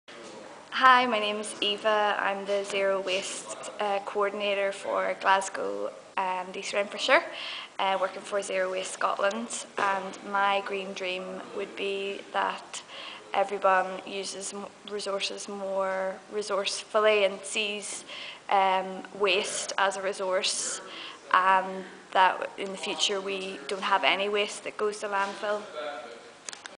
Eva's Zero Waste is Green Dreaming
Eva's Green Dream is that sources are used more sustainably so that there is no waste for landfills anymore.